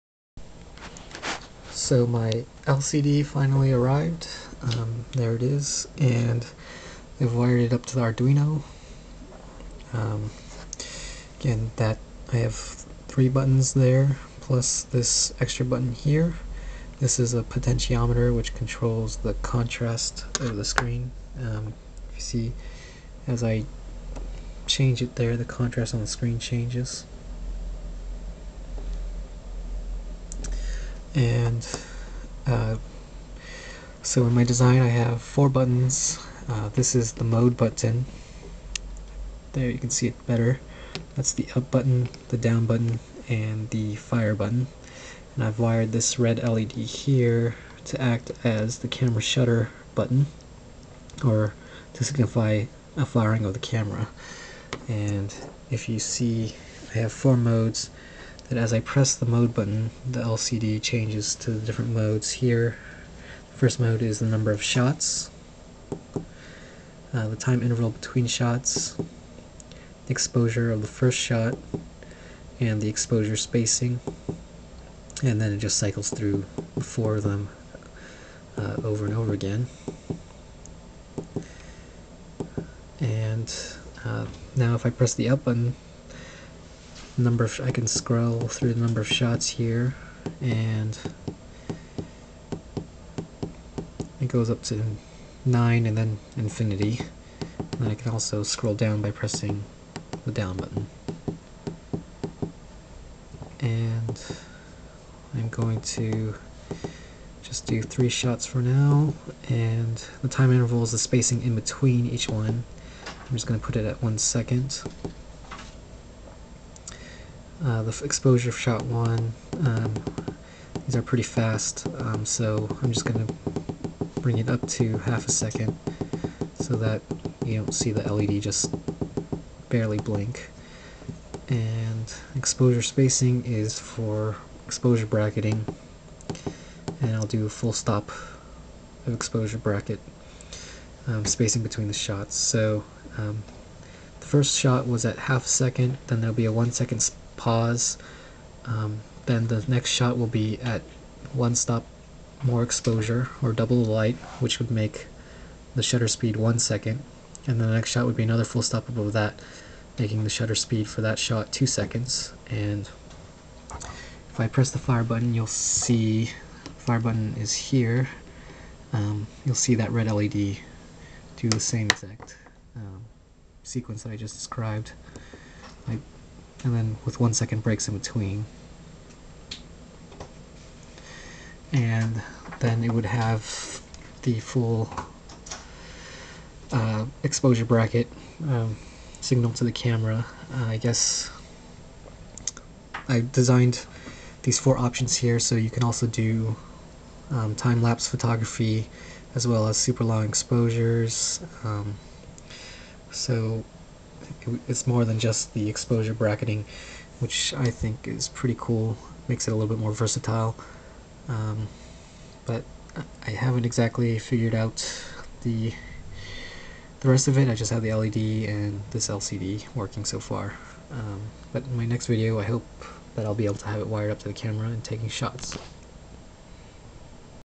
intervalometer on arduino 4
Installed an LCD on the intervalometer prototype